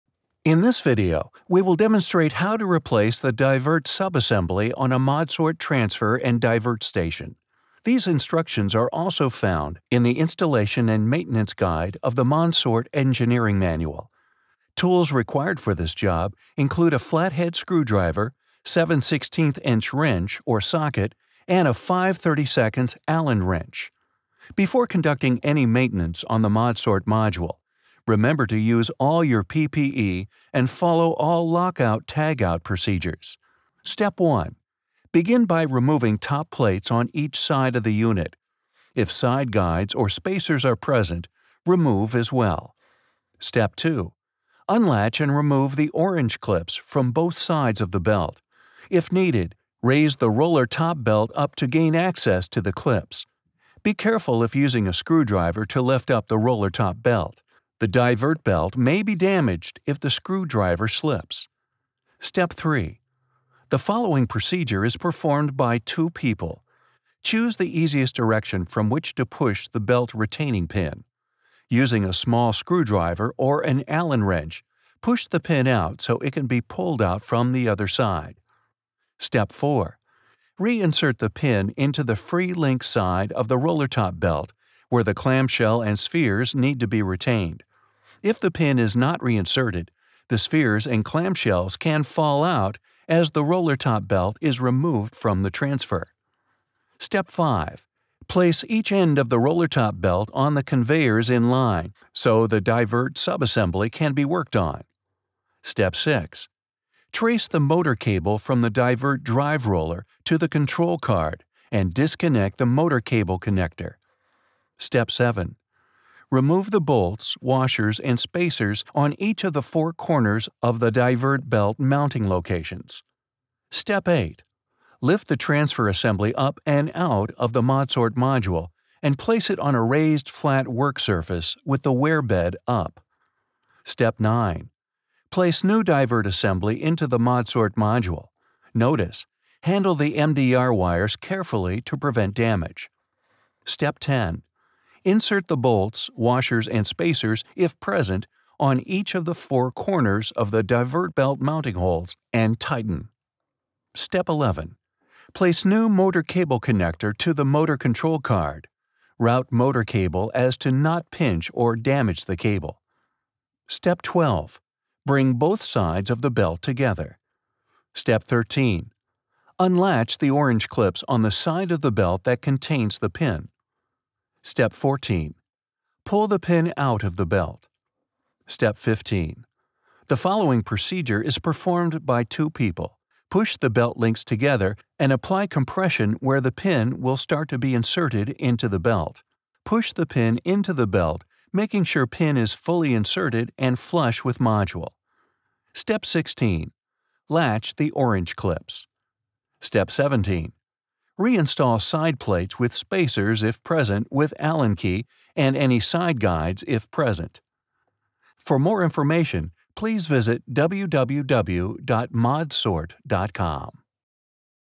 Divert Sub Assembly Replacement on the System Plast® Modsort® Transfer and Divert Module.
What if one integrated module could easily divert, transfer and sort multiple package types? The patented ModSort® Divert and Transfer Module can be added to an existing or new conveyor system and utilizes industry-standard 24V connections to help ease installation in your application.

for additional information.

MCV22002E